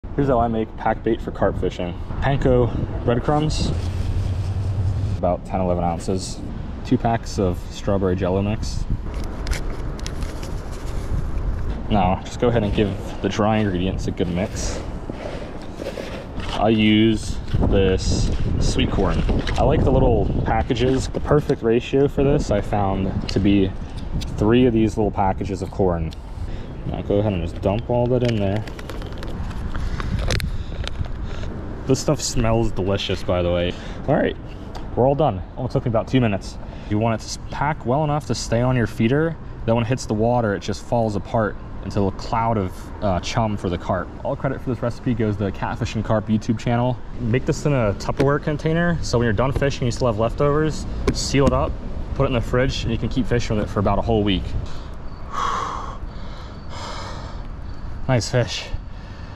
Best and Easiest Way to Make Carp Bait! I Swear by this Stuff
If you want to catch a ton of carp make this bait!! You can pack it around your sinker or a special feeder which you can find off amazon/other retailers. Get out there and do some carp fishing! Its a ton of fun!